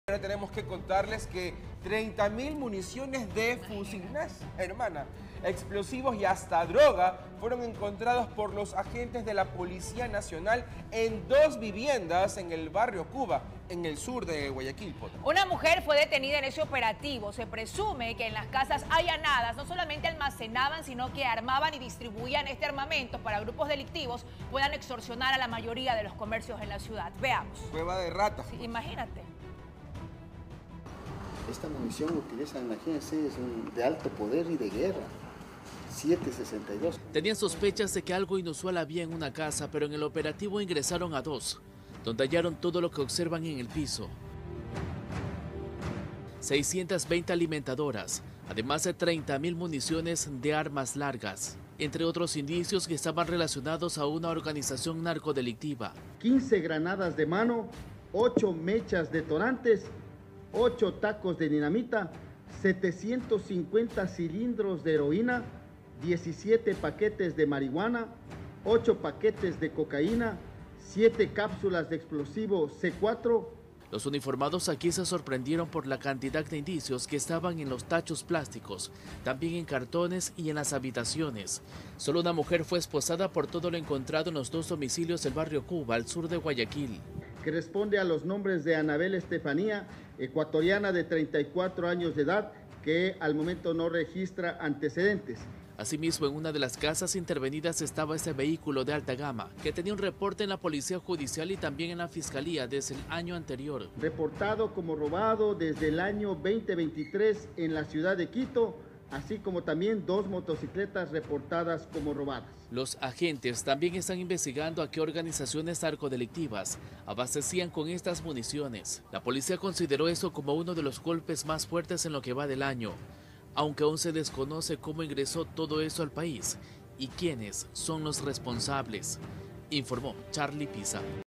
Operativo al sur de Guayaquil terminó con armamento decomisado
Aproximadamente 30 mil municiones, explosivos e incluso sustancias ilícitas fueron encontrados por los agentes de la Policía Nacional gracias a los trabajos de inteligencia que se hicieron en Barrio Cuba. Una mujer fue detenida en el operativo